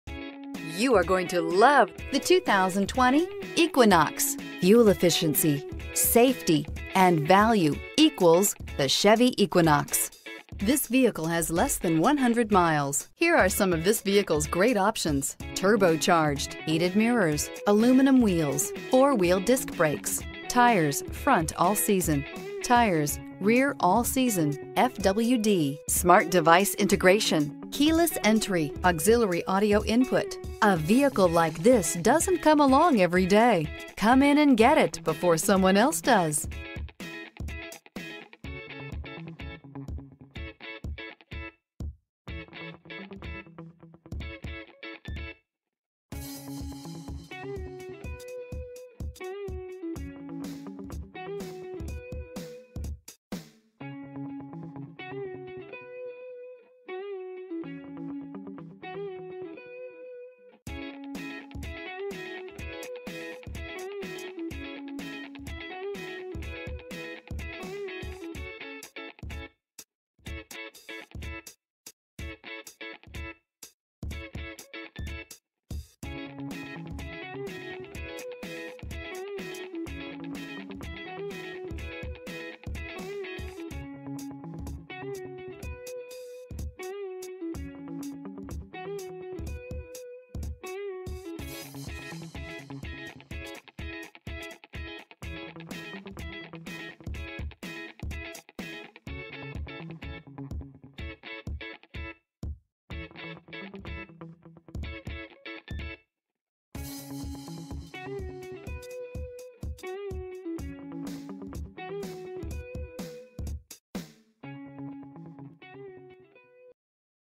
2020 Chevrolet Equinox V20895
2020 CHEVROLET EQUINOX Villa Park, IL
Stock# V20895

Castle Chevrolet
400 E Roosevelt Road

Delivers 31 Highway MPG and 26 City MPG! This Chevrolet Equinox delivers a Turbocharged Gas I4 1.5L/92 engine powering this Automatic transmission.

Test drive this must-see, must-drive, must-own beauty today at Castle Chevrolet, 400 E Roosevelt Rd, Villa Park, IL 60181.